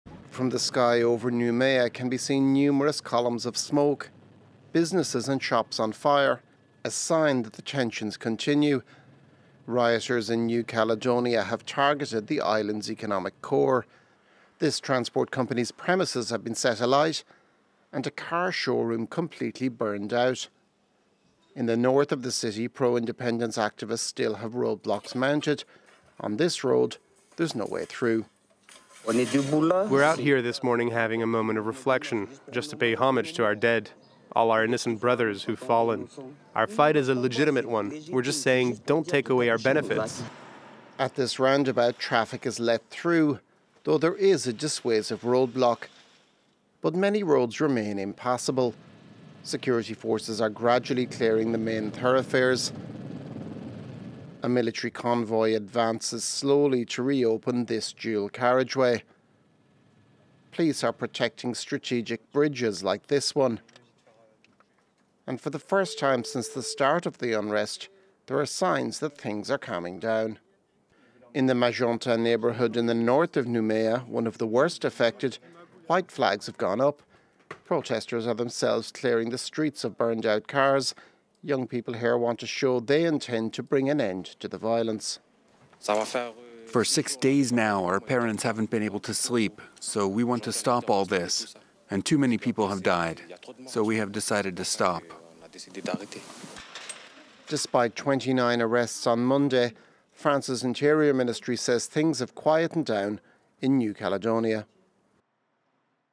New Caledonia riots: Tensions remain high • FRANCE 24 English
The Pacific territory of 270,000 people has been in turmoil since May 13, when violence erupted over French plans to impose new voting rules that would give tens of thousands of non-indigenous residents voting rights. But for the first time since the start of the unrest, there are signs that things are coming down.